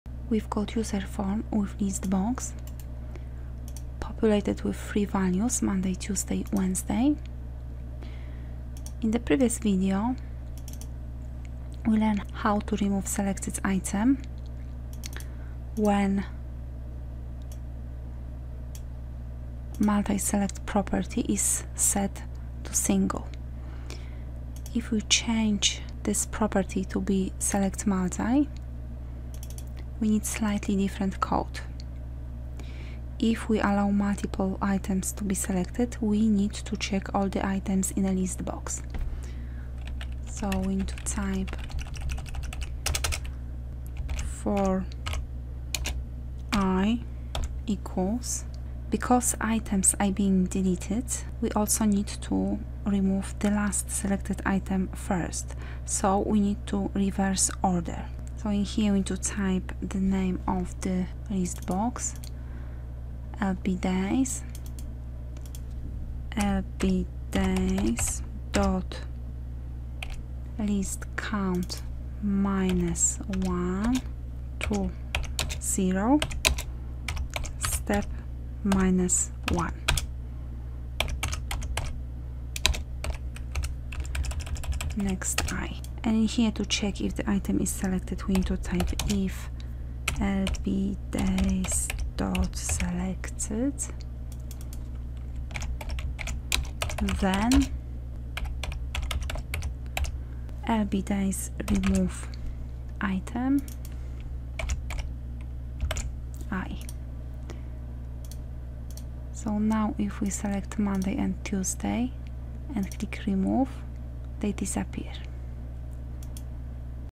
Excel VBA UserForm Listbox Remove Item (Multiselect)
Excel VBA UserForm Listbox Learn how to remove item

The source code used in this video:

Private Sub cbRemove_Click()

For i = lbDays.ListCount - 1 To 0 Step -1
If lbDays.Selected(i) Then lbDays.RemoveItem (i)
Next i

End Sub

Private Sub UserForm_Initialize()

With lbDays
.AddItem "Monday"
.AddItem "Tuesday"
.AddItem "Wednesday"
End With

End Sub